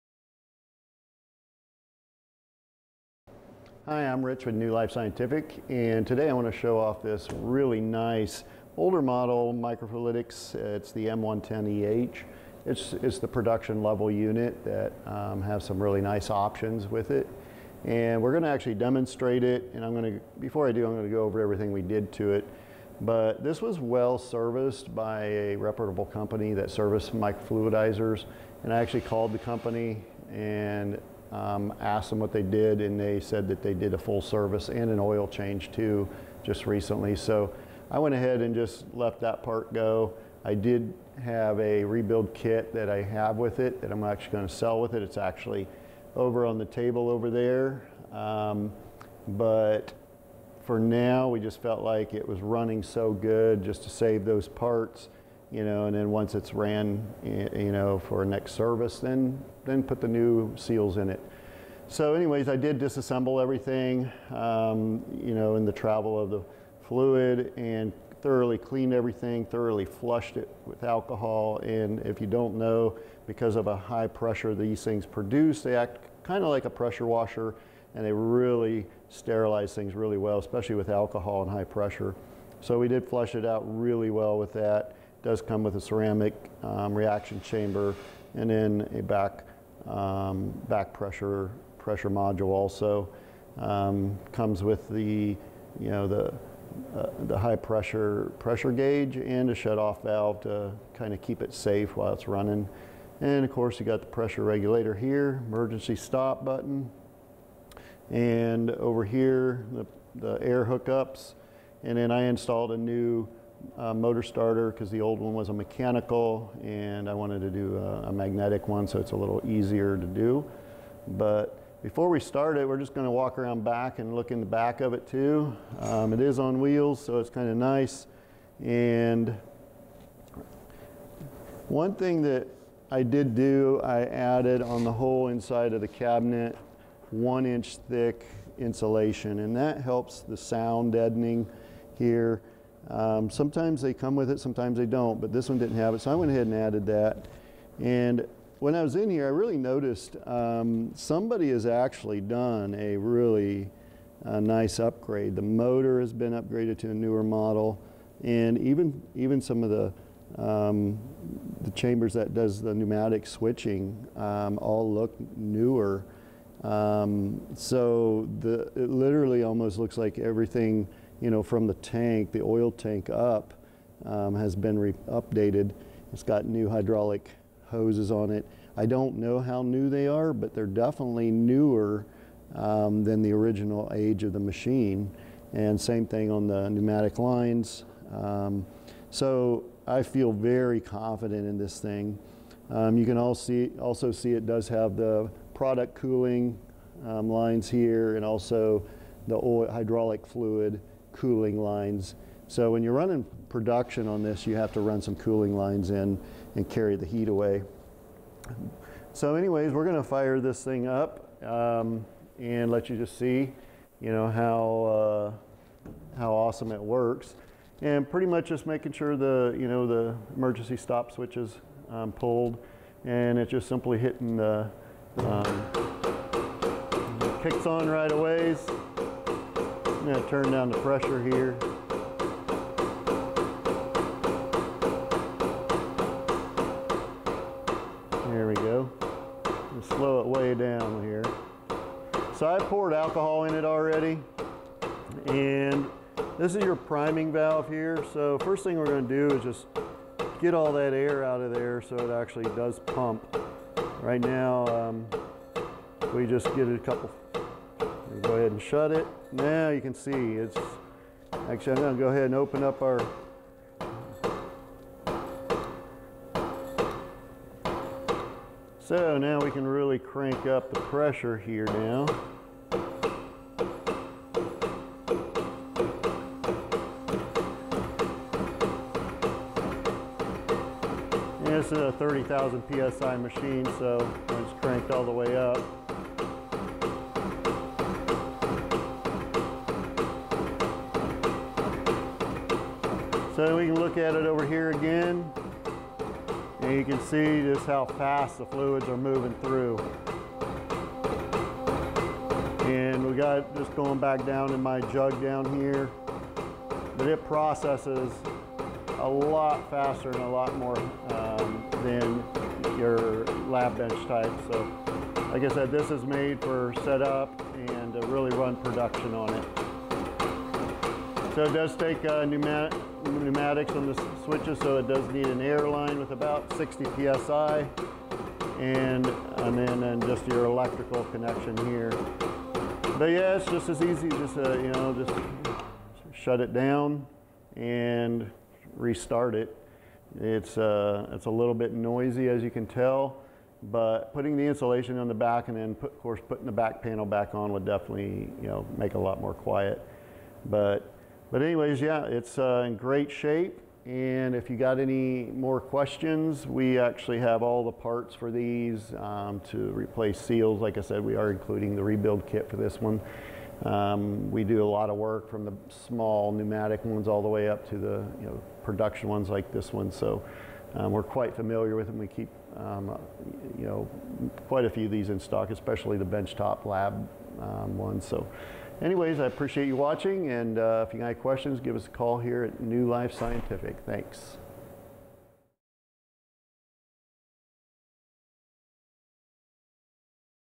Microfluidics M-110EH Microfluidizer Processor Homogenizer Cell Disrupter
Microfluidics M-110EH Microfluidizer Processor Homogenizer Cell Disrupter Overview

Welcome to New Life Scientific! In this video, Rich introduces the M-110EH Microfluidics unit, a robust production-level machine designed for high-efficiency processing. This video will demonstrate its impressive capabilities and highlight the extensive servicing and upgrades it has undergone.

Key Features of the M-110EH Microfluidics
- High-Performance Production Unit: Capable of reaching up to 30,000 PSI, making it ideal for extensive production needs.
- Comprehensive Servicing: Recently serviced by a reputable company with an oil change and a thorough cleaning to ensure optimal performance.
- Enhanced Components: Features a ceramic reaction chamber, back pressure module, high-pressure gauge, and a new magnetic motor starter for improved operation.
- Upgraded Parts: Comes with new hydraulic hoses, pneumatic lines, and insulation for sound reduction, enhancing both safety and user experience.

Demonstration Overview
In this video, we’ll:
- Walk through the setup process and the features of the M-110EH.
- Demonstrate the operation of the unit, including priming and adjusting pressure settings.
- Showcase the efficiency and speed of the microfluidics process compared to typical lab bench models.

Operation Insights
- Watch as we illustrate how to effectively operate the unit, including the emergency stop feature and the importance of cooling lines during production.
- Learn about the necessary pneumatic connections and how easy it is to shut down and restart the machine.